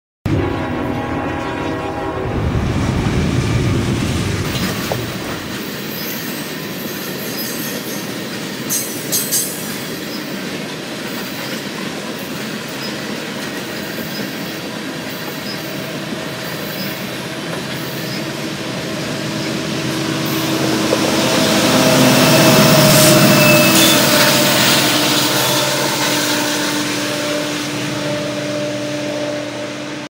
NCT Short #2: CP Heritage on the 650
CP heritage unit 7013 on the southbound 650 oil train.